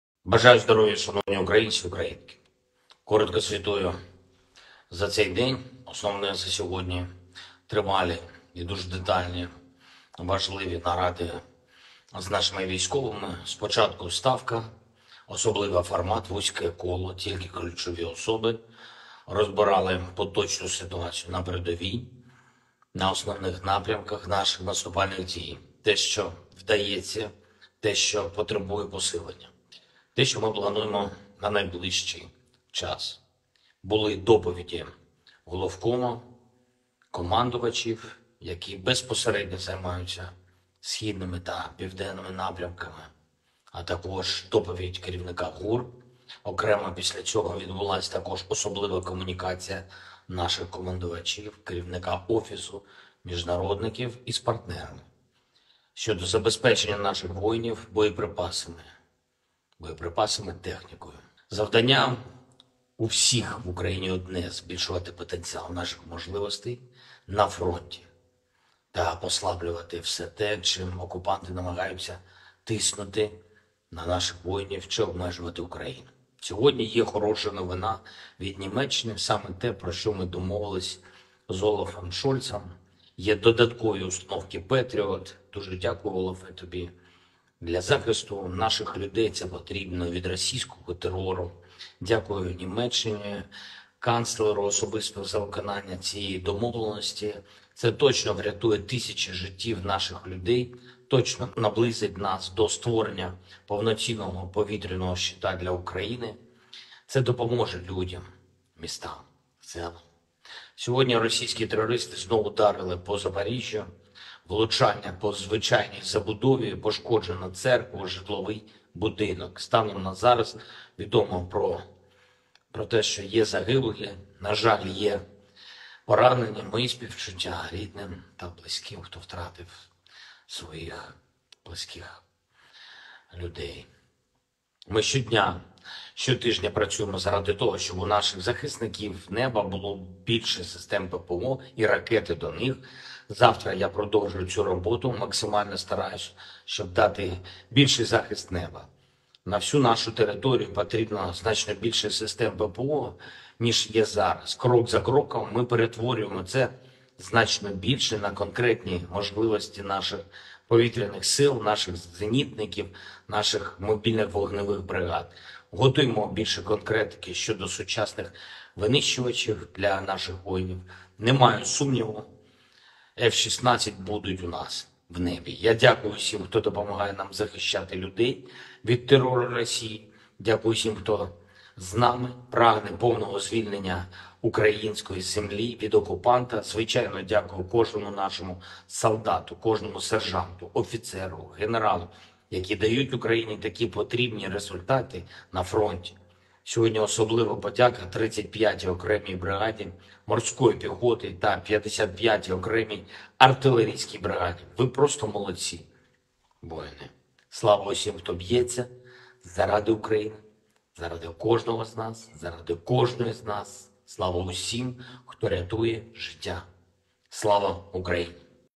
9-8-2023 zelensky daily speech english subtitle
The key thing today is long and very detailed important meetings with our military. First, there was the Staff. A special format - a narrow circle, only key people.

We analyzed the current situation at the front, the main directions of our offensive actions. What is going well. What needs to be reinforced. What we are planning for the near future.

There were reports from the Commander-in-Chief, the commanders directly responsible for the eastern and southern directions, as well as the report from the Chief of the Main Intelligence Directorate.

Separately, there was also a special communication between our commanders, the Head of the Office, international relations officials and partners to provide our warriors with ammunition and equipment.

Everyone in Ukraine has the same task: to increase the potential of our capabilities at the front and to weaken everything the occupiers are trying to use to put pressure on our warriors or restrict Ukraine.

Glory to all those who are fighting for the sake of Ukraine, for the sake of each of us!
Glory to all who save lives!